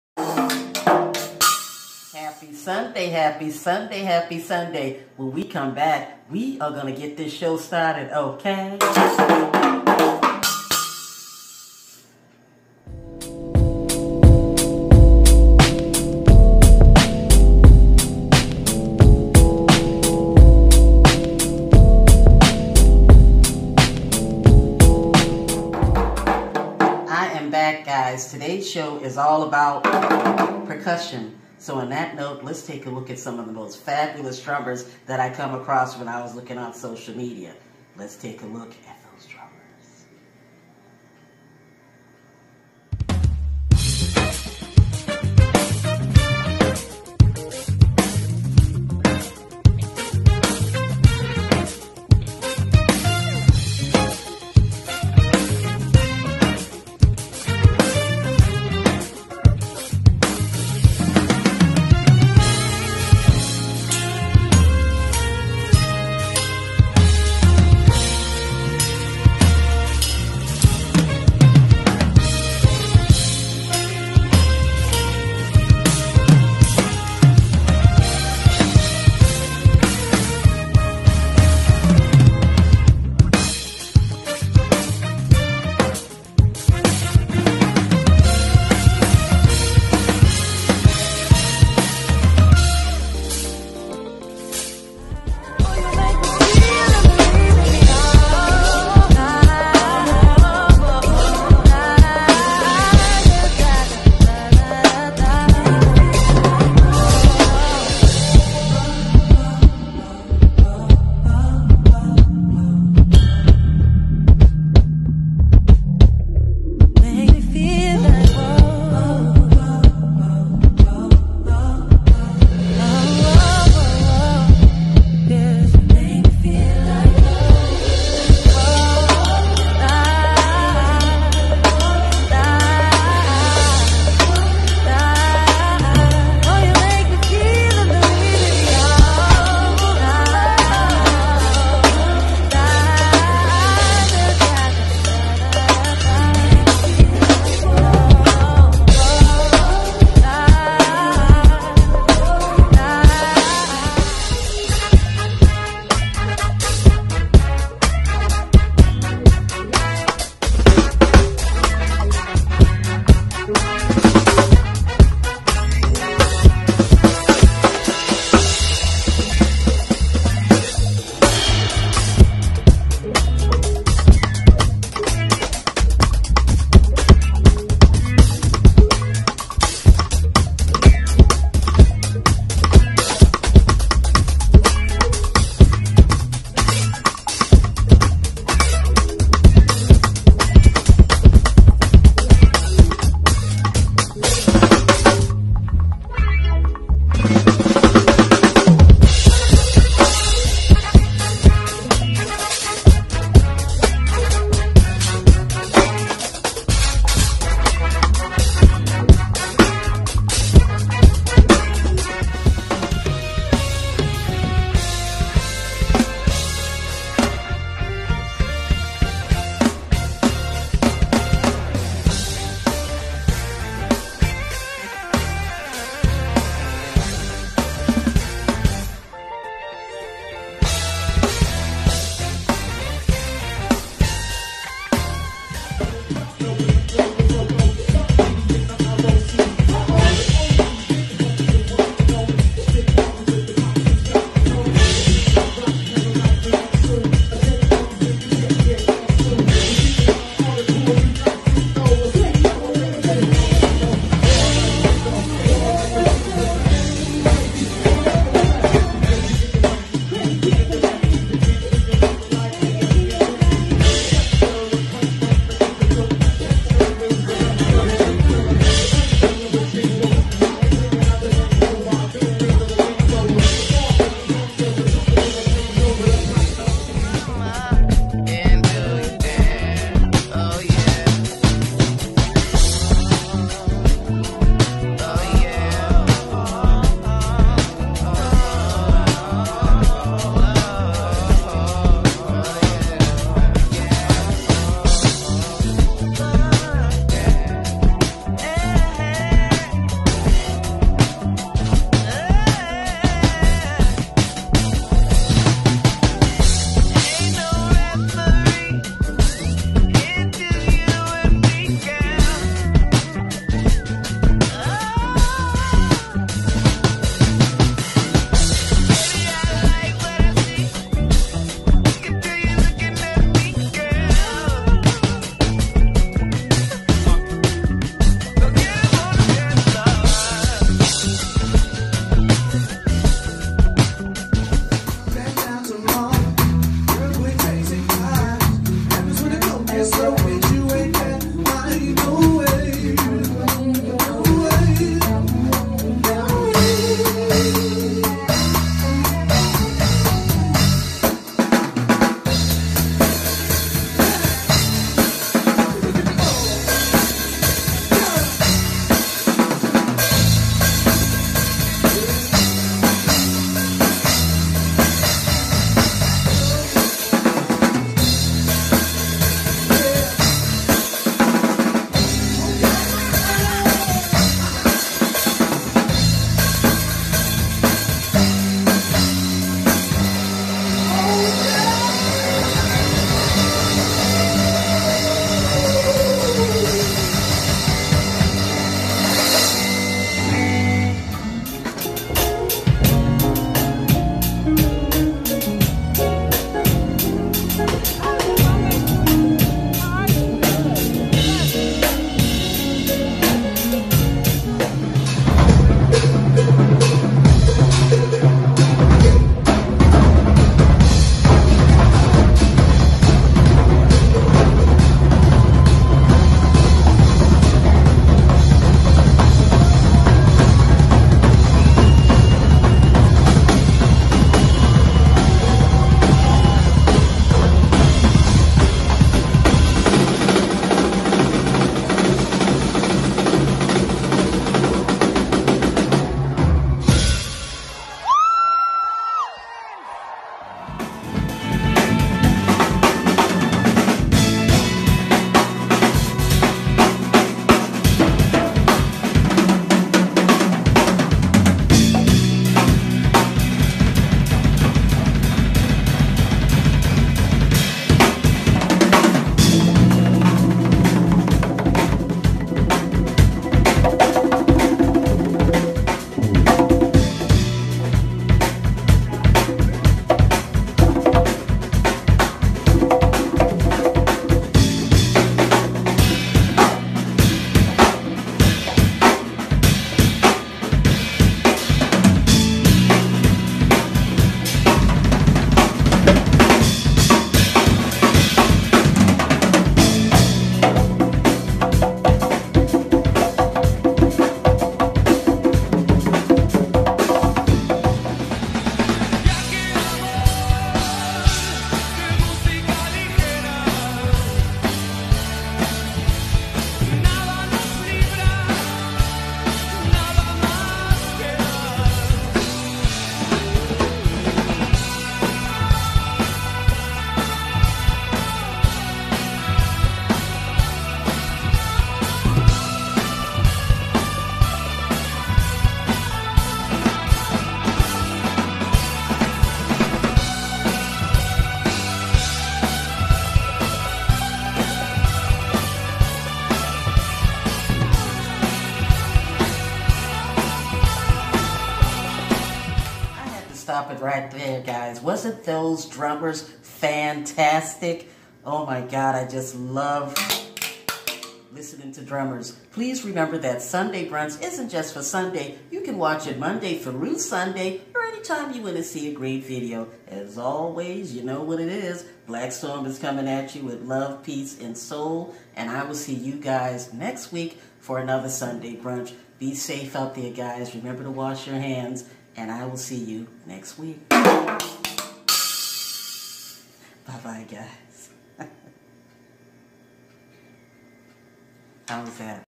Percussion Drums
Happy Sunday sorry for the late upload. This is my first attempt editing from a different software due the the closing of the studio and I don't have adequate access to my editing computer. Please sit back and enjoy some awesome drumming from the pocket queen, jonathan newman, duckie drums, kitrich, the name is duncan, johnawooton, and minifairhurst. I own not copyrights. Pleases be safe during this safe distancing trying time. Please remember to wash your hands and be blessed.